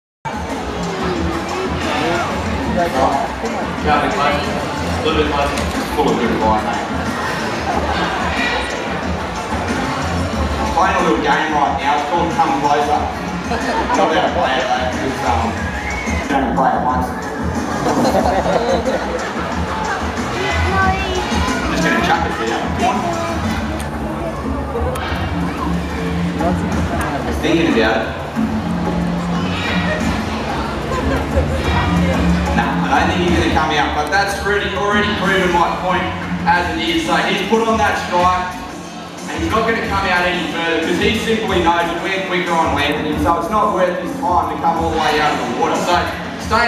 Monty
He was pretty lazy, and didn't want to come all the way out of the water, so just sat with his tail in the water, lol.

Lighting was super weird, because the show was later than usual, and I sat in a weird spot so I couldn't see what I was filming super well because of the glare on my camera screen, but this isn't awful.